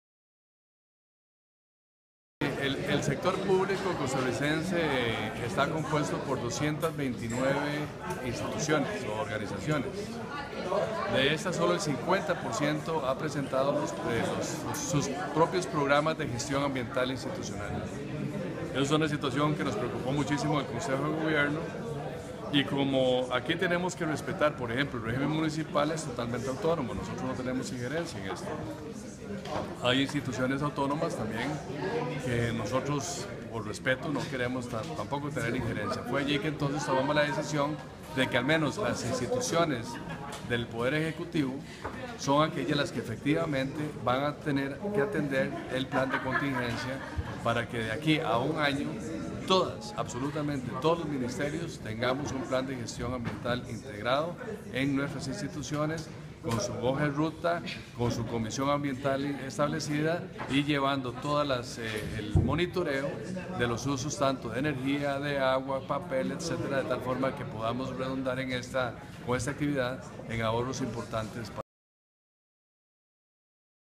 Ministro de Ambiente y Energía, Edgar Gutiérrez
De las 229 instituciones públicas la mitad no cuenta con planes de gestión ambiental